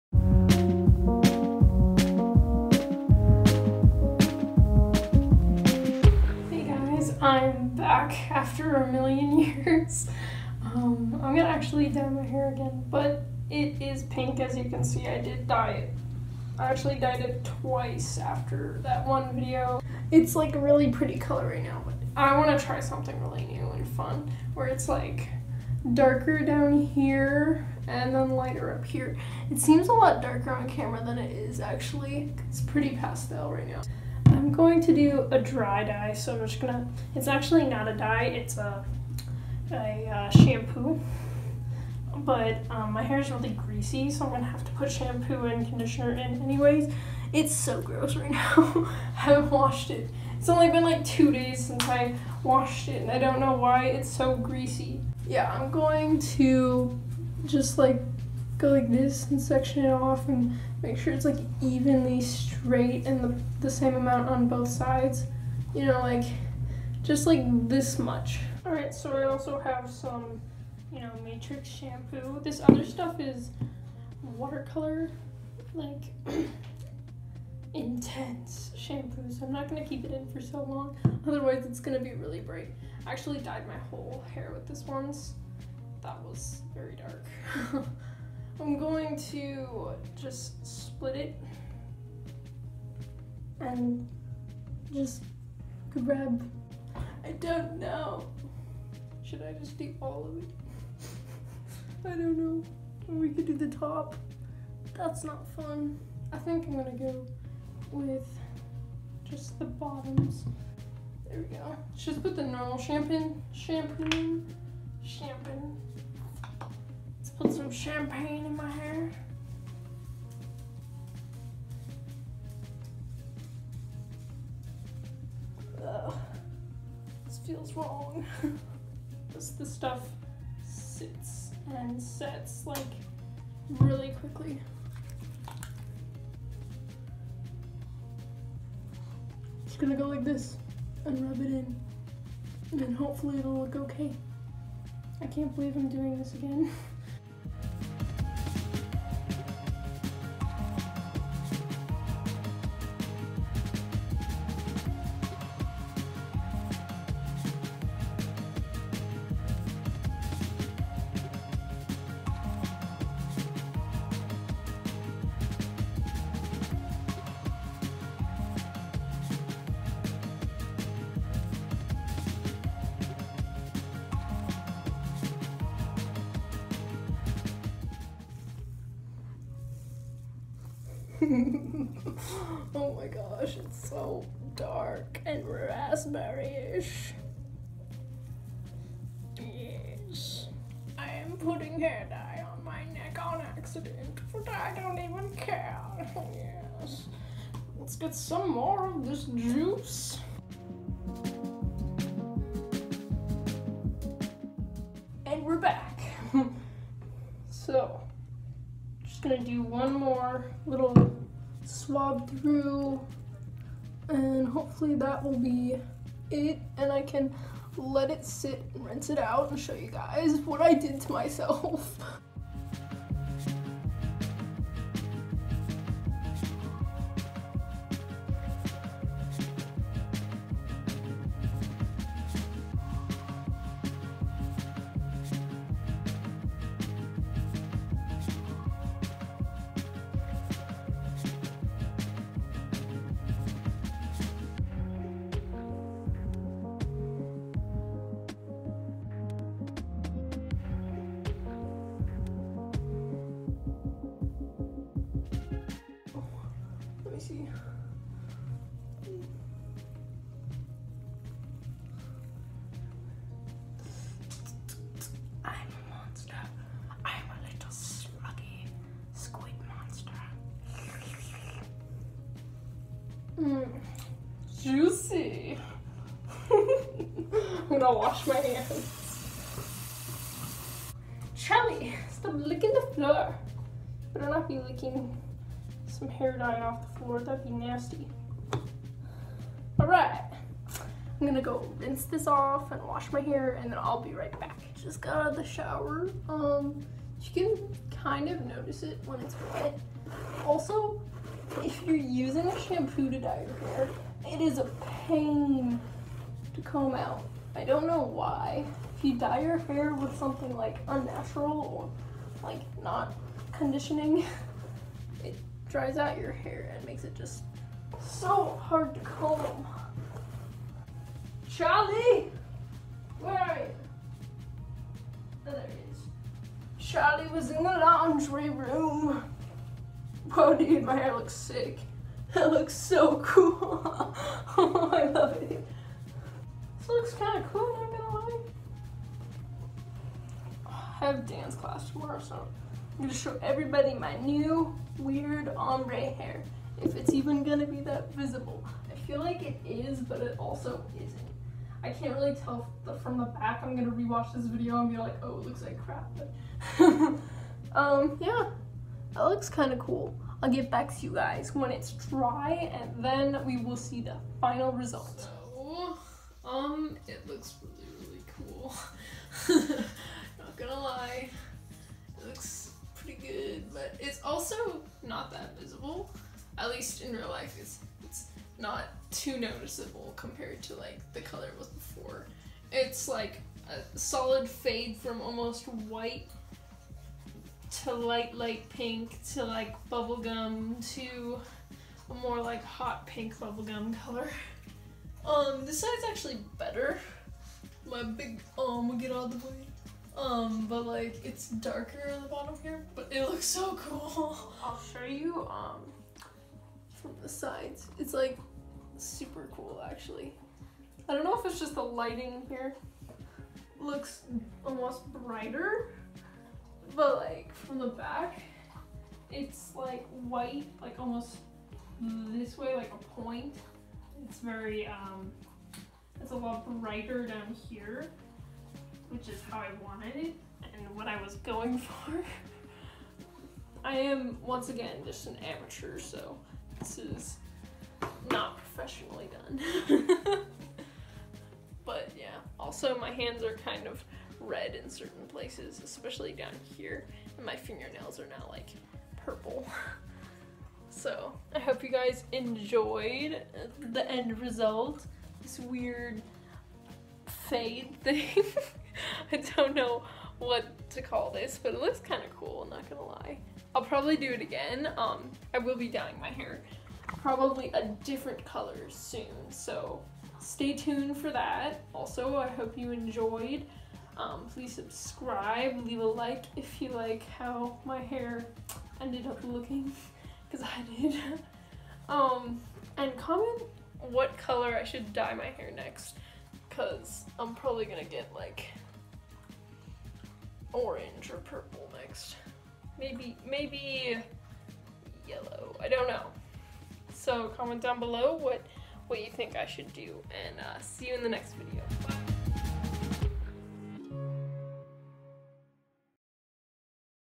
Dyeing My Hair A Pink Ombre
I dyed my hair like I told you guys I would, but it took me a long time to post. I tried a pink ombre look, and I think it looks cool.